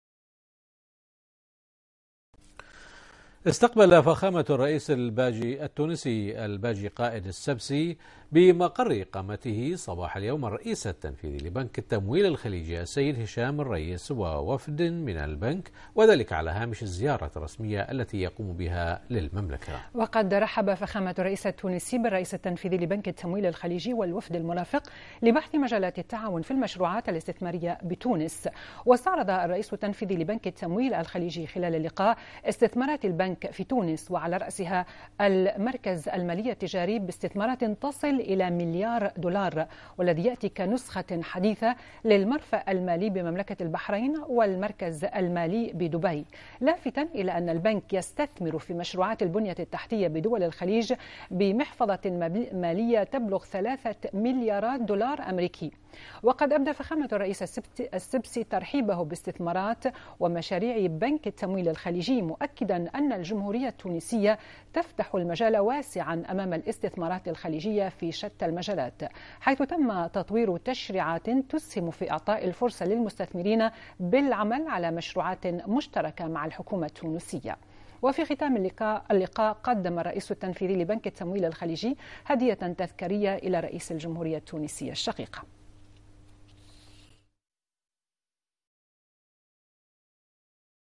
البحرين: رئيس جمهورية تونس يستقبل الرئيس التنفيذي لبنك التمويل الخليجي
المنامة في 28 يناير / بنا / استقبل فخامة الرئيس الباجي قائد السبسي رئيس الجمهورية التونسية الشقيقة بمقر اقامته صباح اليوم الرئيس التنفيذي لبنك التمويل الخليجي السيد هشام الريس ووفد من البنك، وذلك على هامش الزيارة الرسمية التي يقوم بها للمملكة.